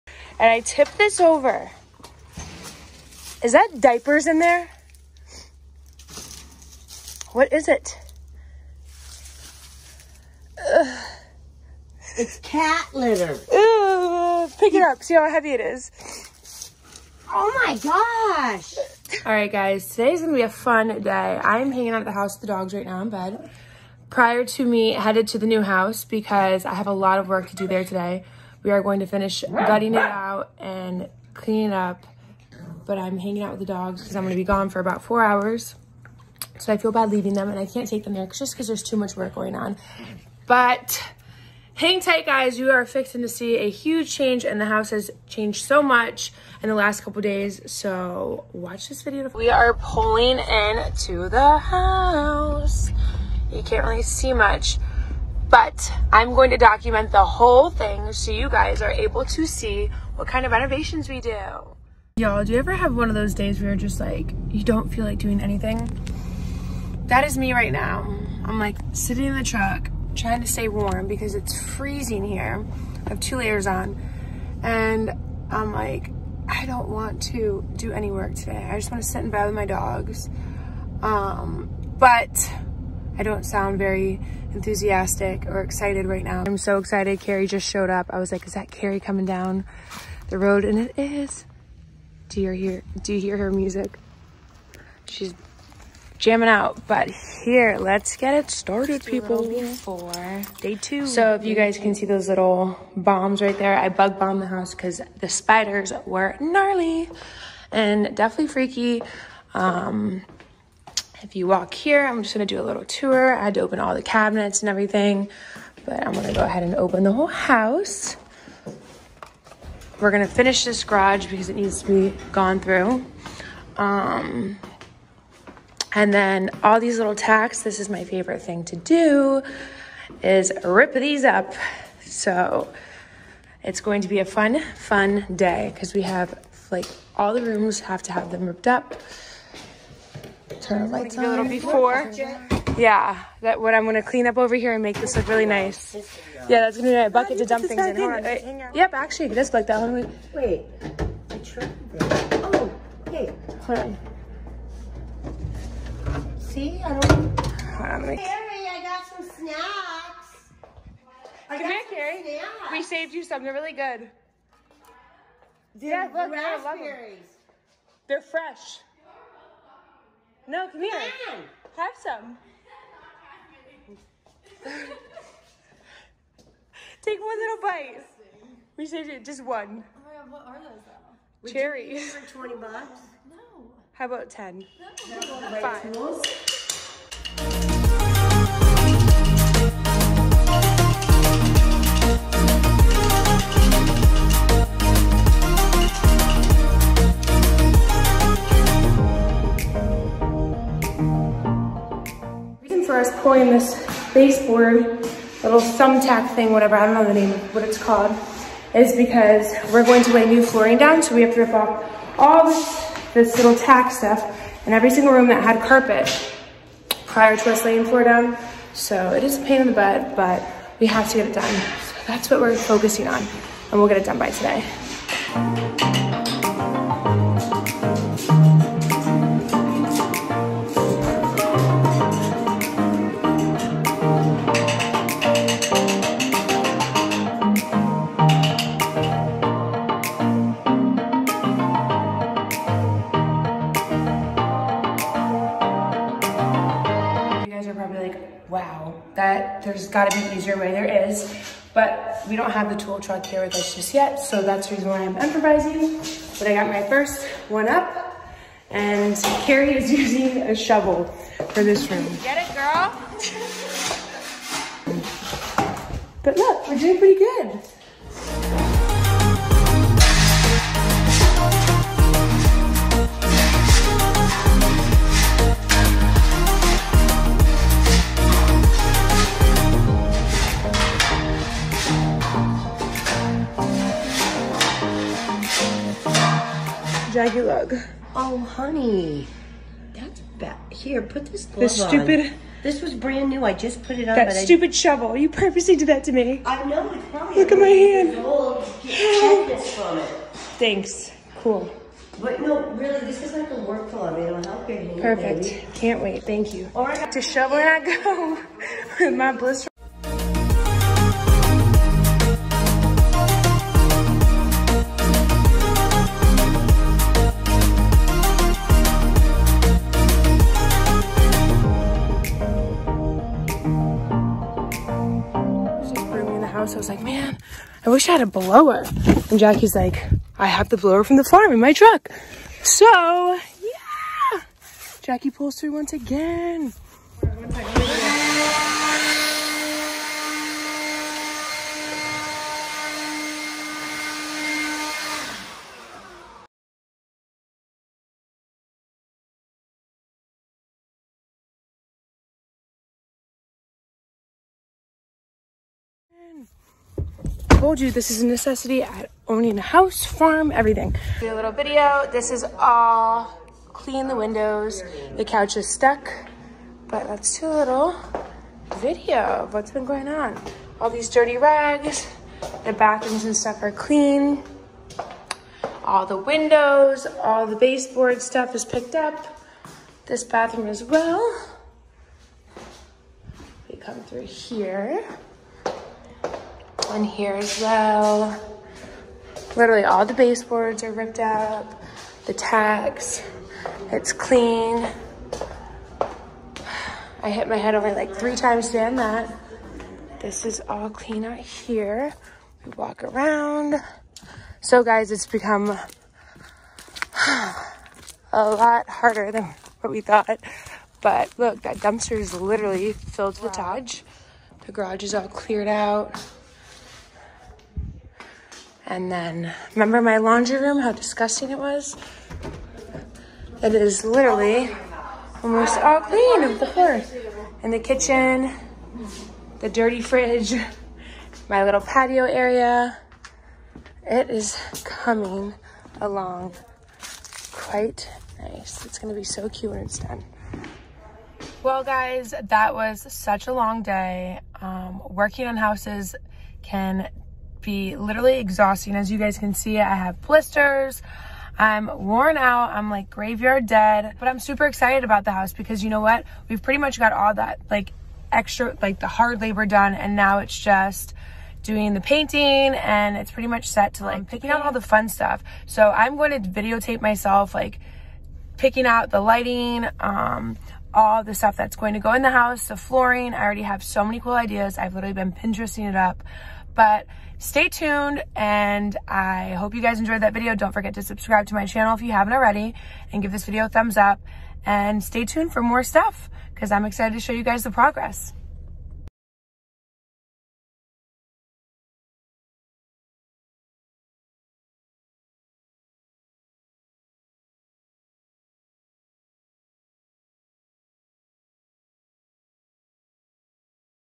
Renovating My New HORSE PROPERTY! | PART 2
I bought a new 6 acre horse property with a house and a horse barn that we are going to be renovating!